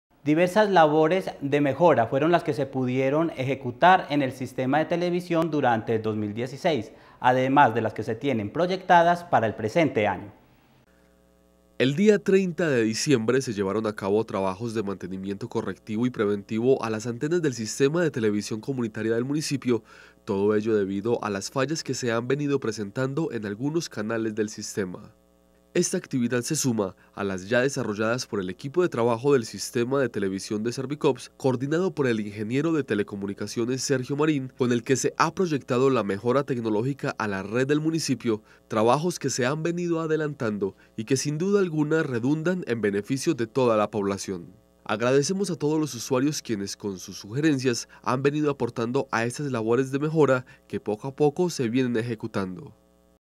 MANTENIMIENTO A LAS ANTENAS DE TV
Diversas labores de mejora al Sistema de Televisión se pudieron ejecutar en el 2016, además de las que se tienen proyectadas para este año.